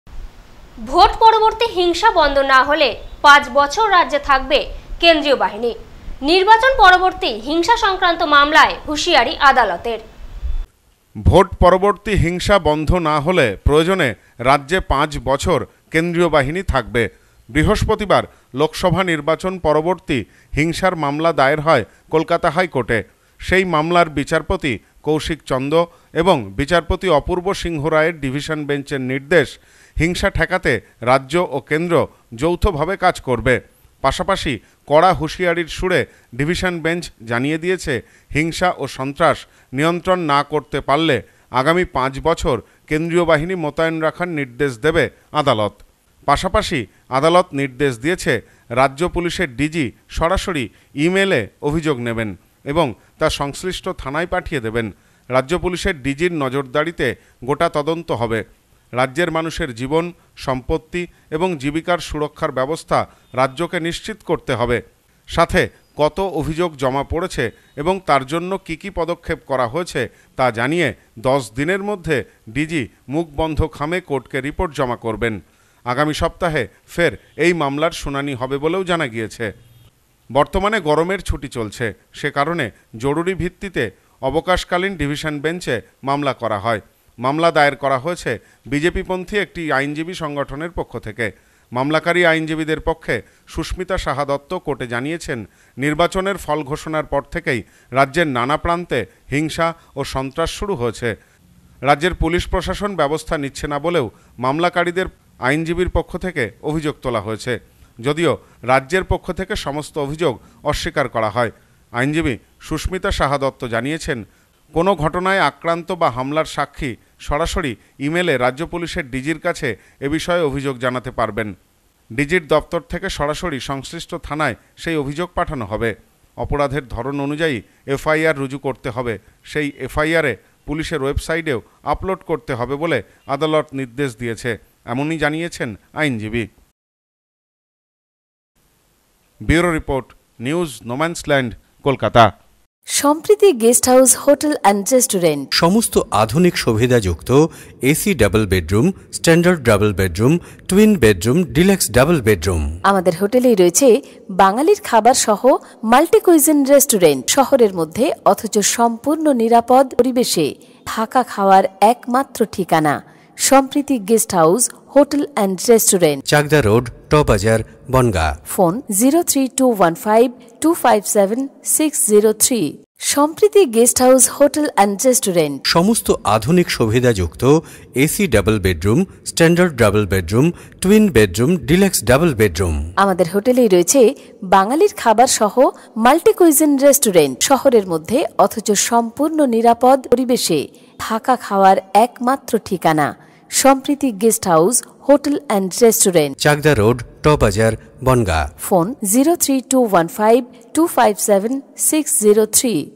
হিংসা বন্ধ না হলে পাঁচ বছর রাজ্যে থাকবে কেন্দ্রীয় বাহিনী: কোর্ট
ভোট পরবর্তী হিংসা বন্ধ না হলে পাঁচ বছর রাজ্যে থাকবে কেন্দ্রীয় বাহিনী। নির্বাচন পরবর্তী হিংসা সংক্রান্ত মামলায় হুঁশিয়ারি আদালতের।